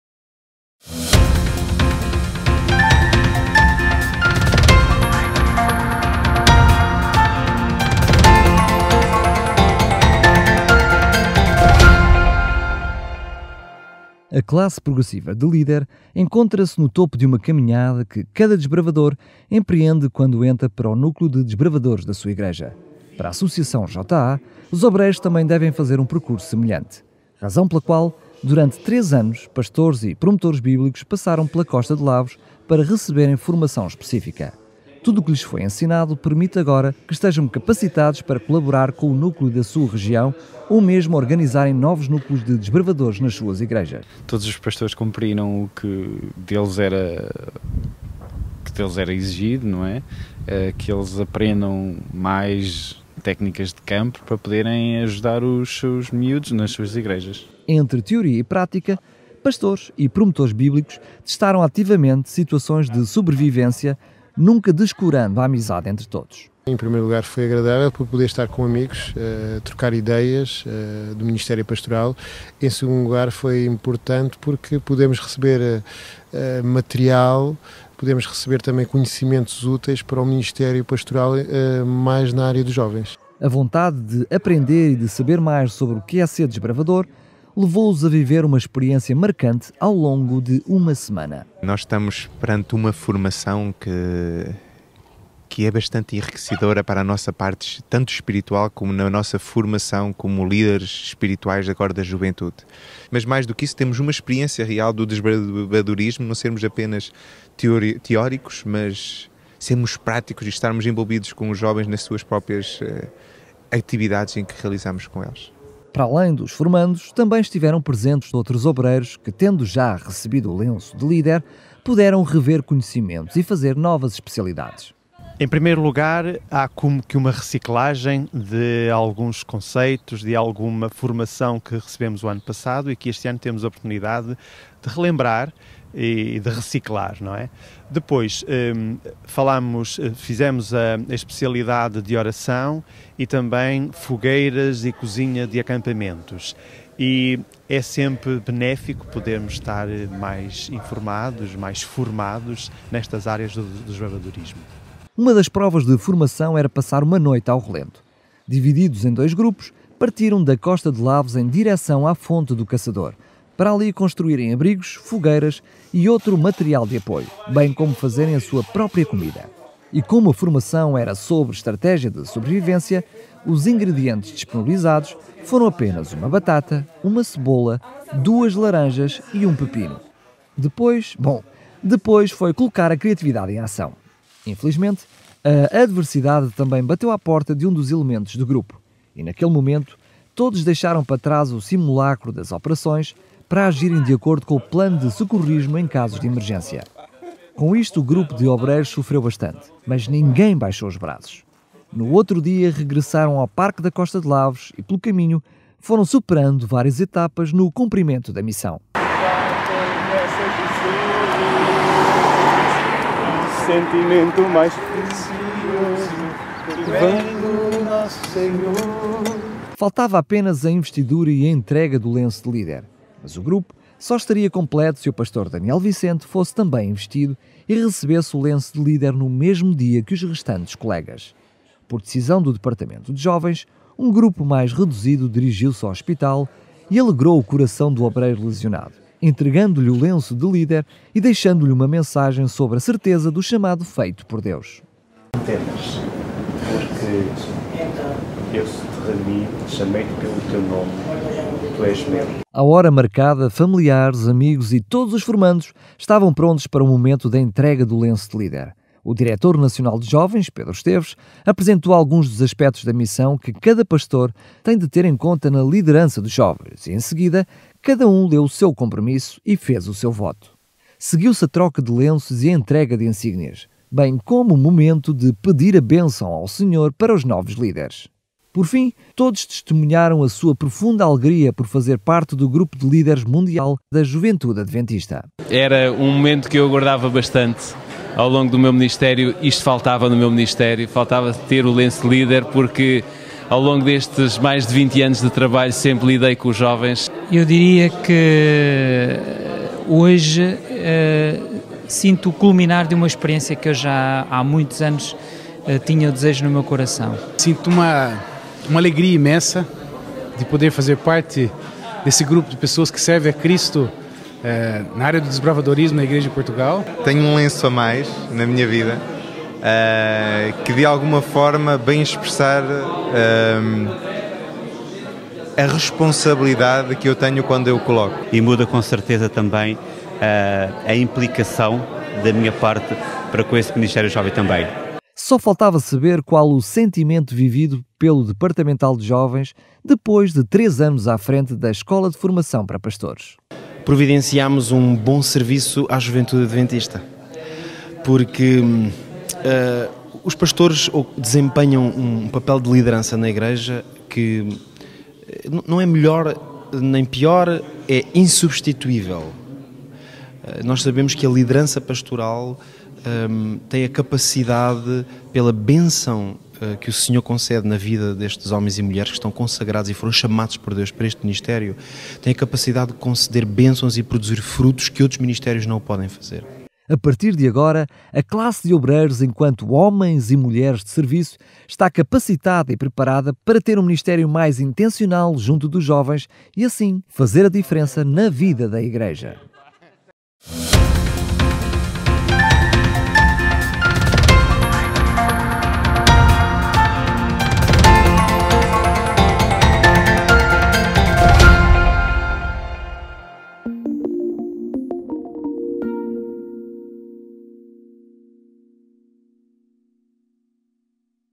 Em Portugal existem seis novos Líderes Pastores
A liderança pastoral tem a capacidade, pela bênção que o Senhor concede na vida de homens e mulheres que estão consagrados e foram chamados por Deus para este ministério, de receber bênçãos e produzir frutos que outros ministérios não podem fazer.
A partir de agora, a classe de obreiros, enquanto homens e mulheres de serviço, está capacitada e preparada para ter um ministério mais intencional junto dos jovens e assim fazer a diferença na vida da igreja.